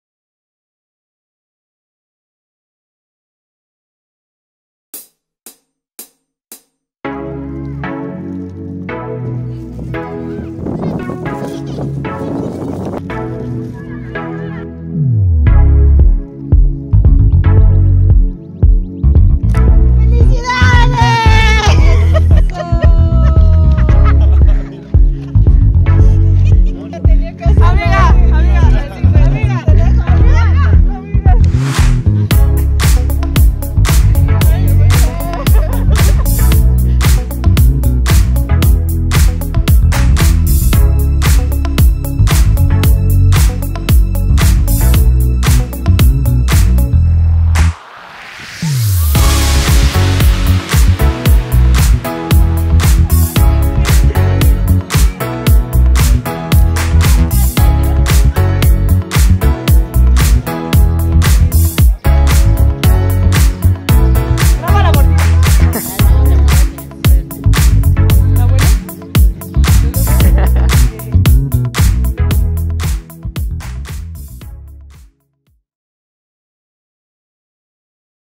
¡Feliz cumpleaños Yanin y Liz!
🥳 ¡Hoy tuvimos celebración y pastelazo por partida doble! 🎂🎂
Nuestras cumpleañeras Yanin Madrid y Liz Ángeles fueron festejadas por las Tuzas tras la práctica con unos deliciosos pasteles. ¡Muchas felicidades a nuestras EmblemáticasTuzas! 🤗🎉